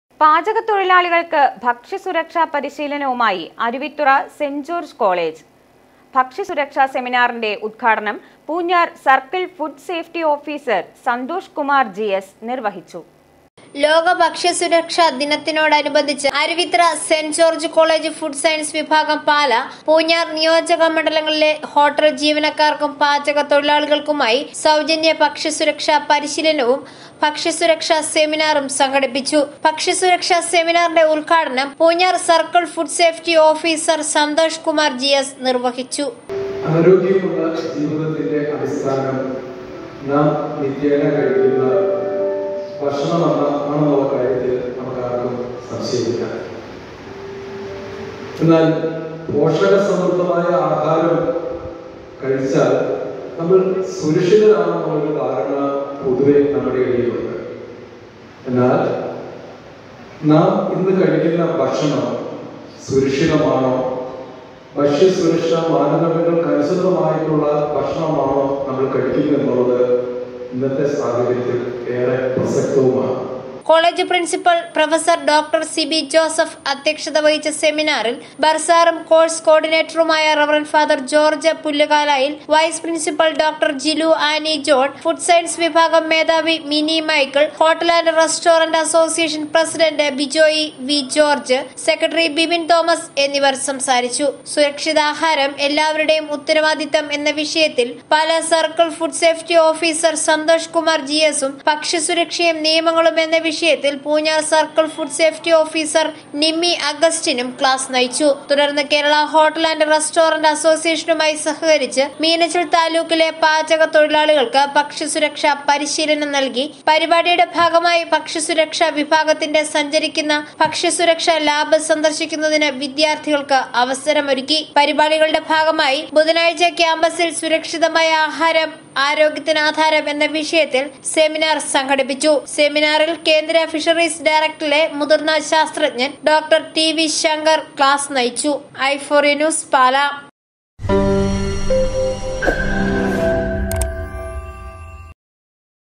പാചക തൊഴിലാളികള്‍ക്ക് ഭക്ഷ്യ സുരക്ഷാ പരിശീലനവുമായി അരുവിത്തുറ സെന്റ് ജോര്‍ജസ്സ് കോളേജ്
പാചക തൊഴിലാളികള്‍ക്ക് ഭക്ഷ്യ സുരക്ഷാ പരിശീലനവുമായി അരുവിത്തുറ സെന്റ് ജോര്‍ജസ്സ് കോളേജ്.സംഘടിപ്പിച്ചു.ഭക്ഷ്യ സുരക്ഷാ സെമിനാറിന്റെ ഉദ്ഘാടനം പൂഞ്ഞാര്‍ സര്‍ക്കിള്‍ ഫുഡ് സെഫ്റ്റി ഓഫീസര്‍ സന്തോഷ് കുമാര്‍ ജി എസ്സ് നിര്‍വഹിച്ചു.